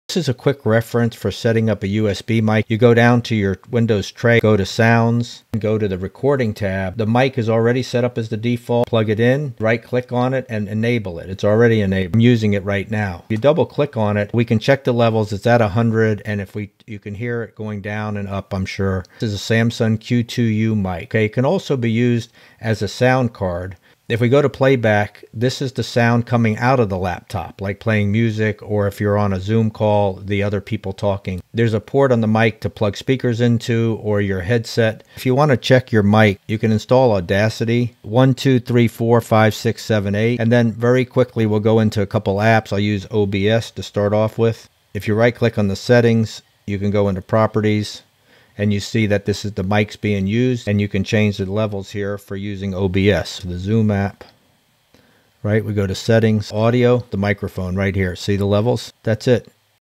Better USB Mic sound on Windows laptop with Samson Q2U Mic app configuration in less than a minute
Configuring a USB microphone on Windows and checking levels on apps such as OBS and Zoom video conferencing in less than a minute; using Samson Q2U mic.